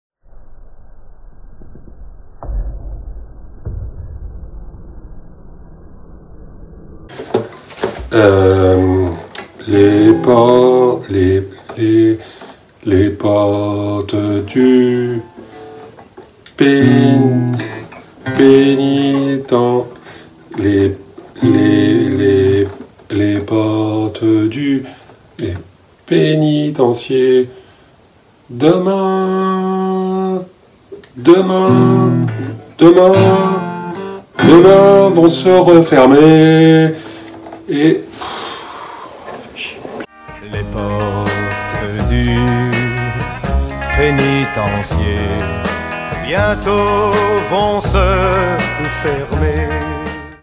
Que fait Marc-André pendant le confinement ? (épisode 1)
Je sais que vous vous posez tous des tas de questions : que fait-il pendant le confinement ? Comment s'occupe-t-il ? Pourquoi a-t-il autant de talent ? Et bien, aujourd'hui, j'ai décidé de me remettre à la guitare et à la chanson. Je vais commencer par quelques grands classiques du genre. La preuve en vidéo...